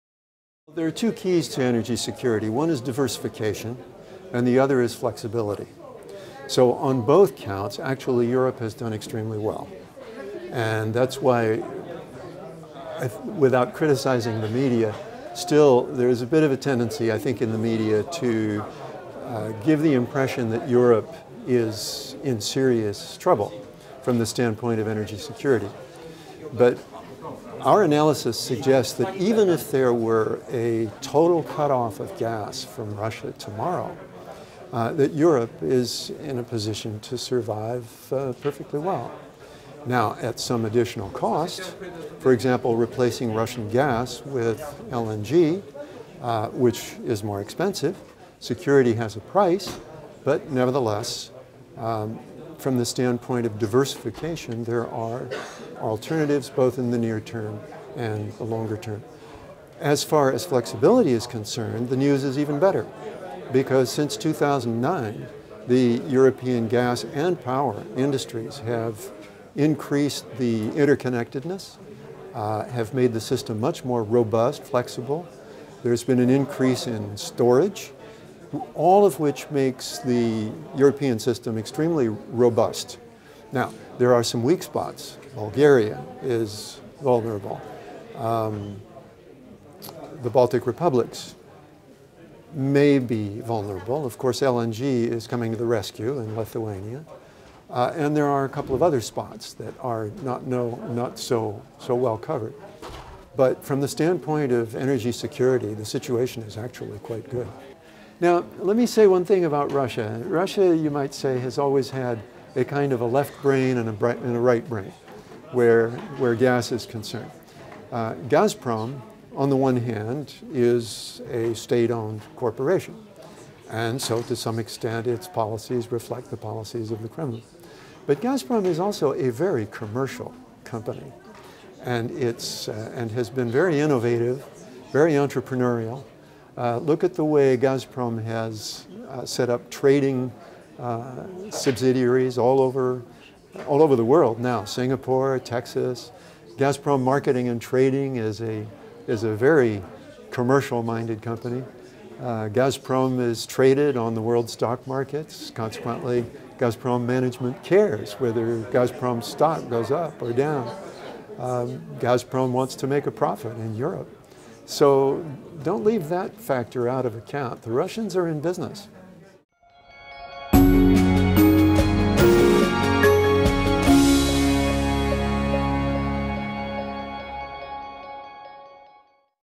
EBRD Annual Meeting: Energy Security and Europe
Achieving the right balance between security, affordability and environmental considerations is one of the most difficult propositions for national governments. Professor Thane Gustafson considers energy security in Europe and the nature of the Russian gas giant, Gazprom.